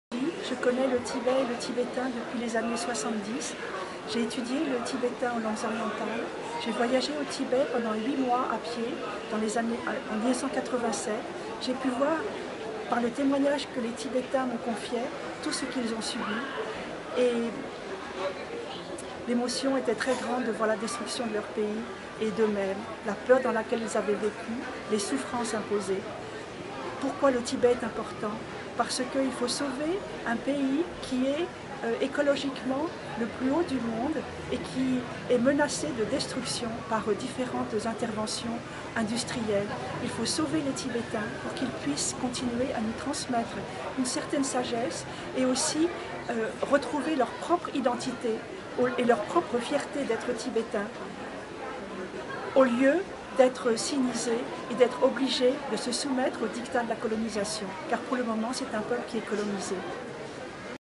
Why Tibet? Pourquoi le Tibet est important - Sylvie
Sylvie explique pourquoi le Tibet est important pour lui et pour l'humanité.

At WhyTibet, people around the world say why Tibet matters to them and humanity. Watch their 60-second videos and add your own.